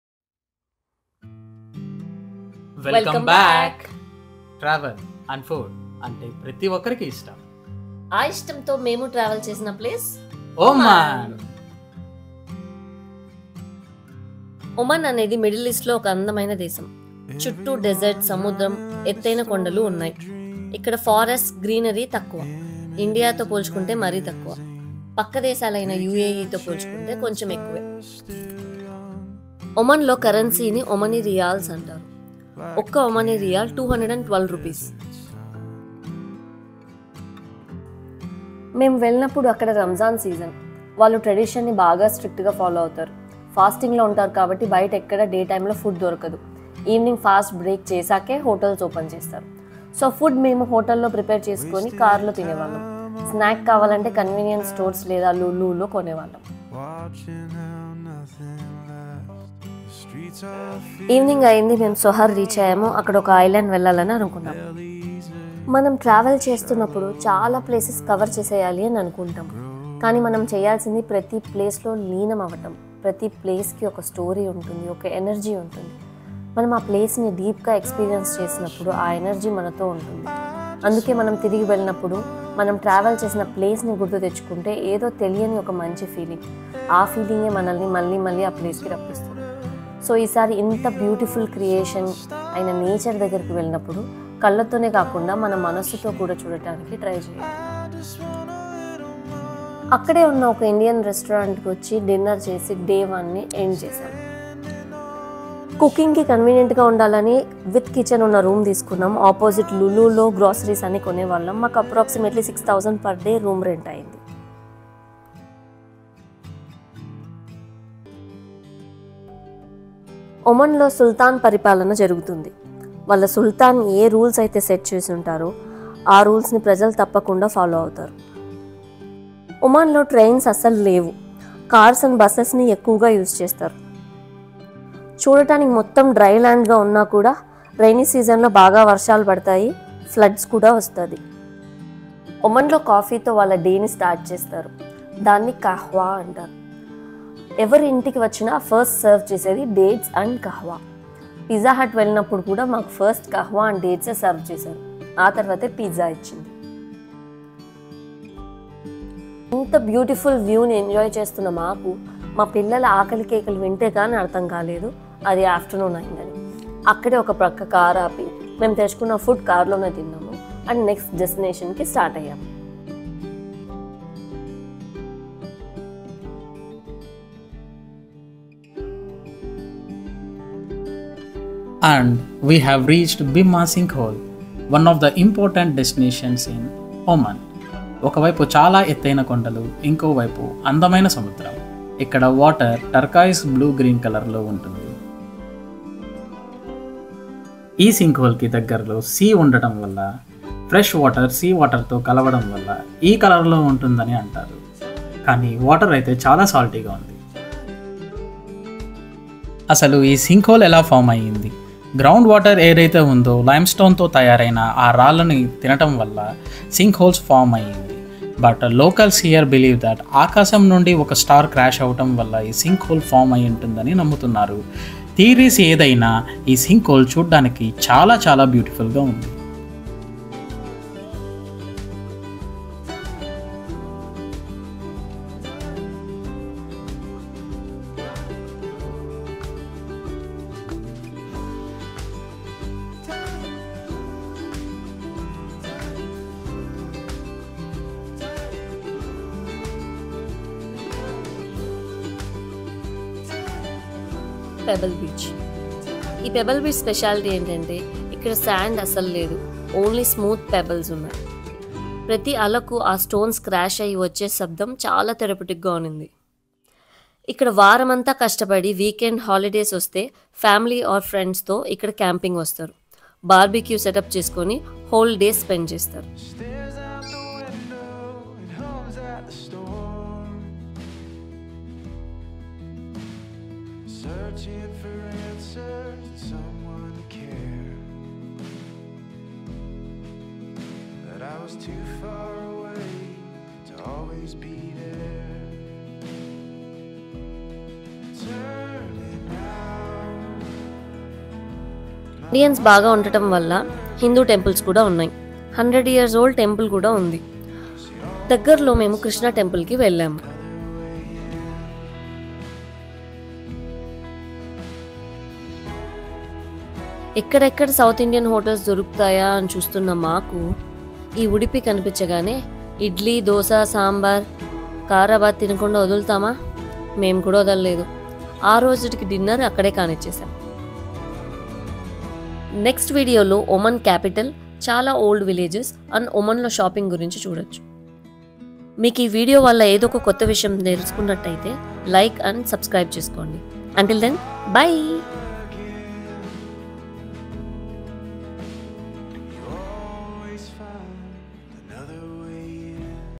Oman Trip vlog || Bimmah Sinkhole Talesofmouni
We have shared you guys our 4 day Oman trip highights  , Oman is a very beautiful country and if you have doubts regarding travel to Oman do comment , We will definitely answer your questions.

మీకు ఒమాన్ కాపిటల్ ఏంటో తెలుసా? తెలిస్తేయ్ కామెంట్ చేయండి .

Music from Uppbeat (free for Creators!)